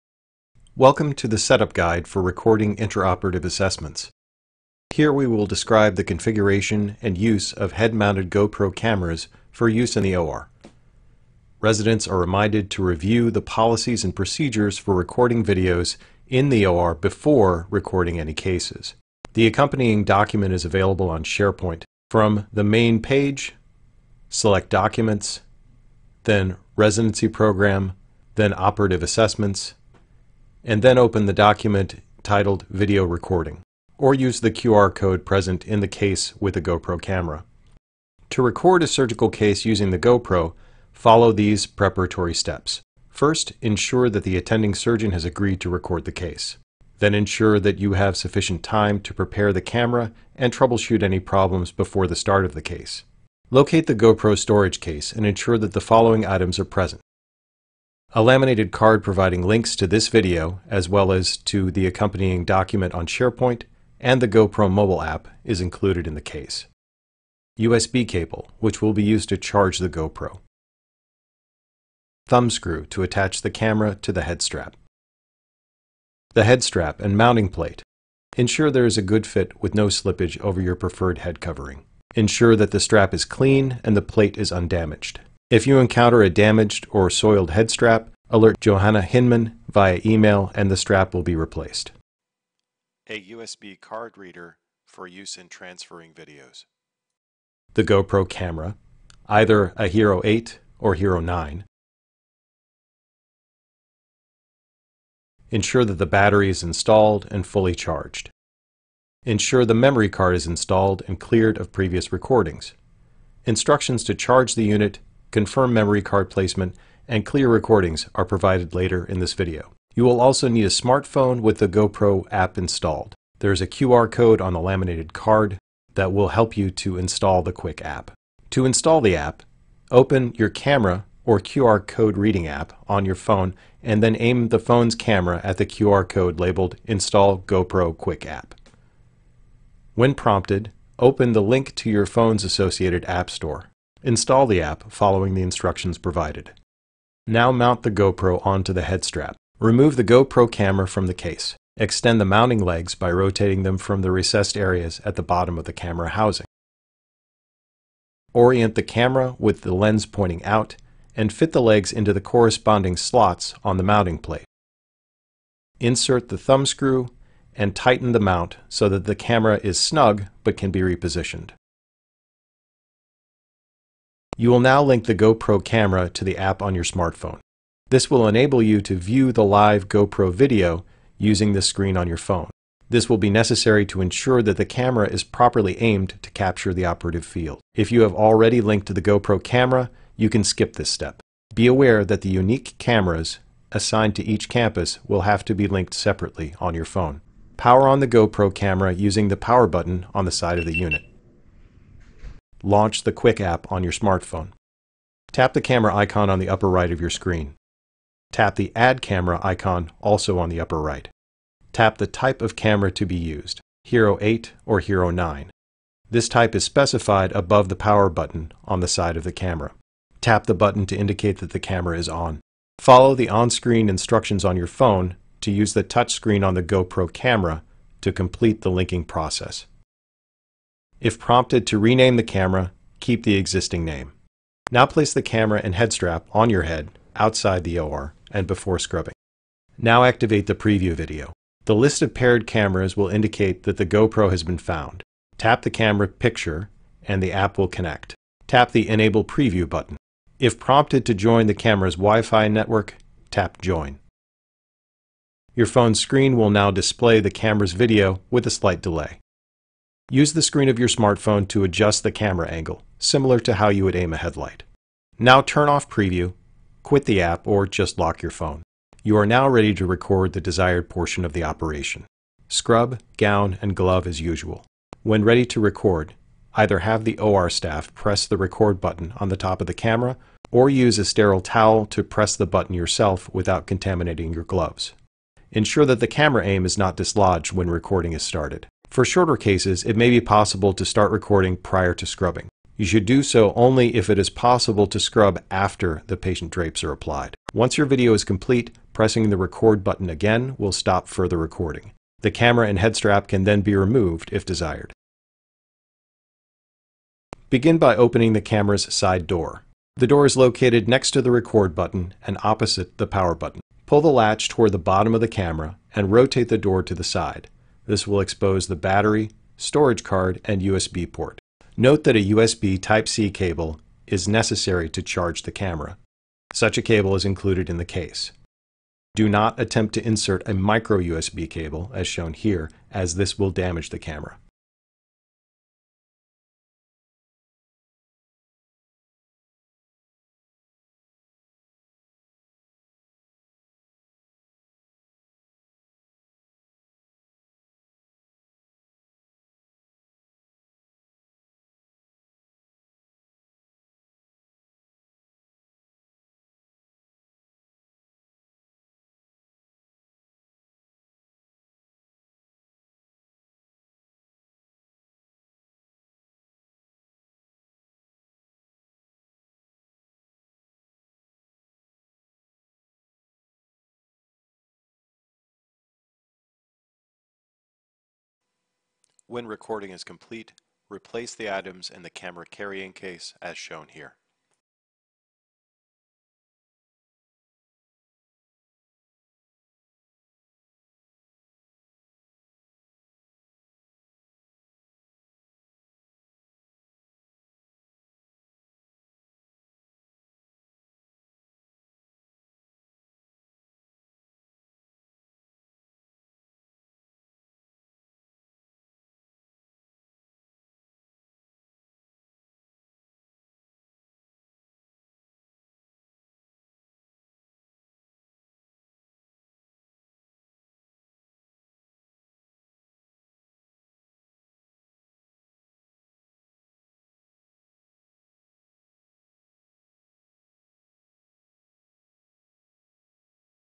Recording Setup
Describes setup for head-mounted GoPro cameras for use in recording cases for subsequent technical evaluation.